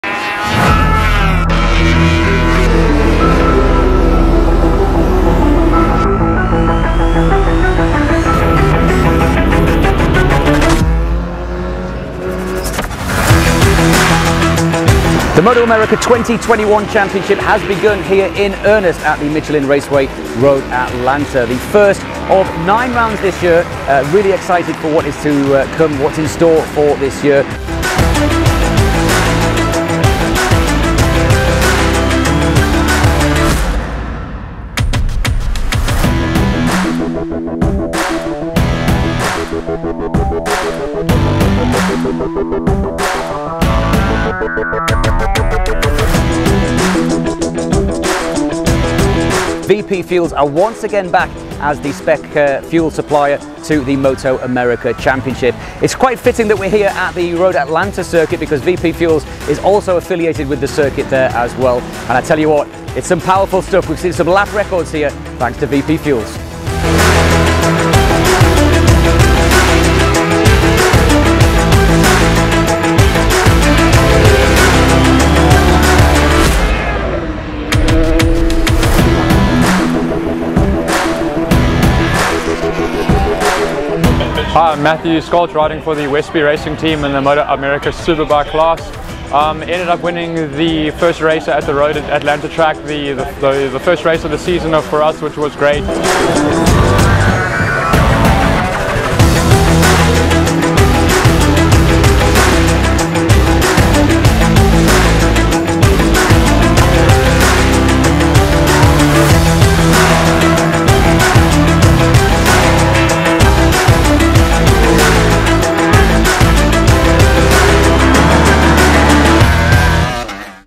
MotoAmerica Road Atlanta Fueled By VP
The first race of the much anticipated 2021 MotoAmerica season kicked off at Road Atlanta for a great weekend full of amazing motorcycle racing action in all classes from Stock to SuperBike and even the full size baggers!